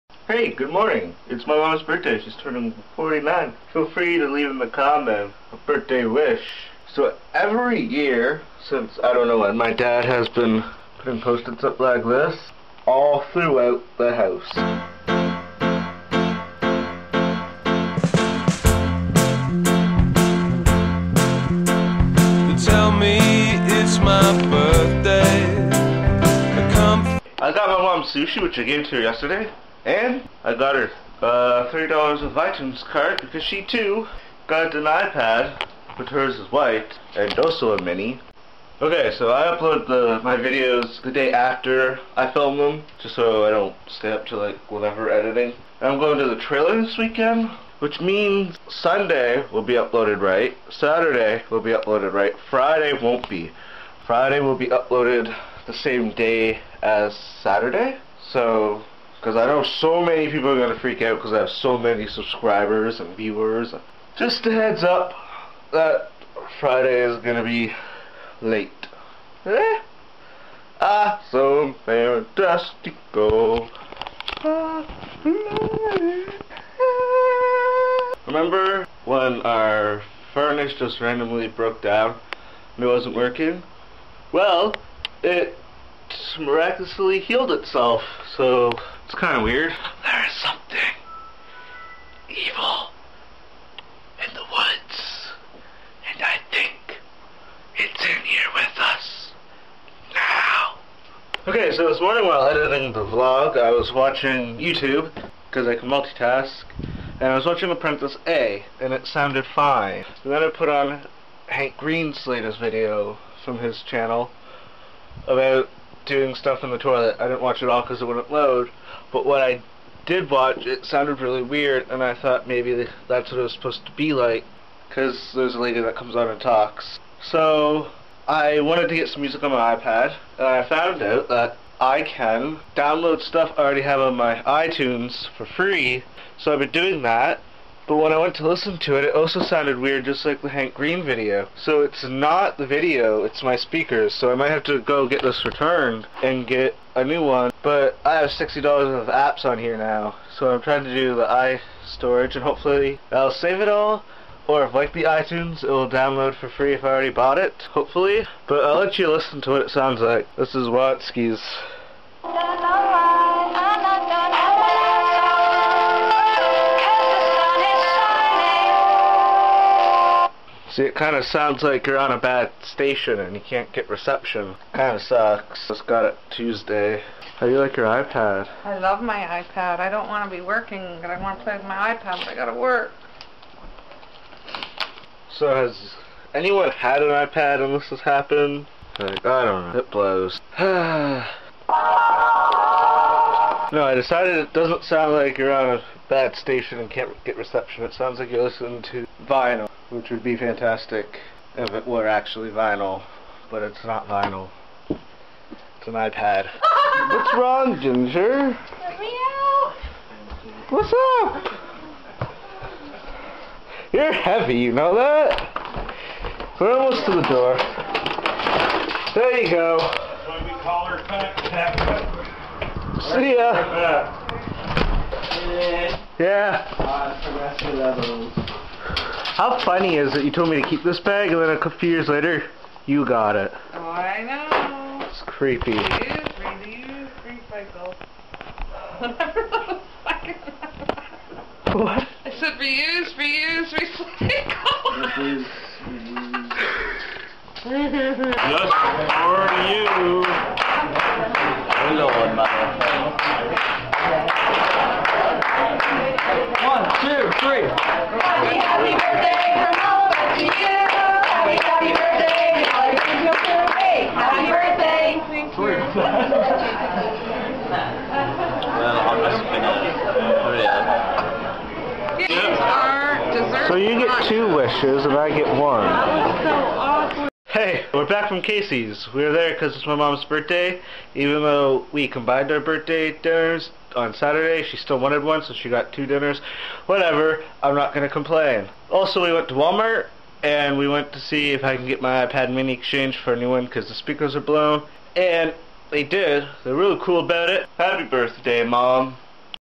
Moms 49th Birthday And First World Problems (02.22.2013 - Day 8)
April 22 2013 Day 8
In which my mom turns 49. a cat comes out of the bag and i suffer from first world problems.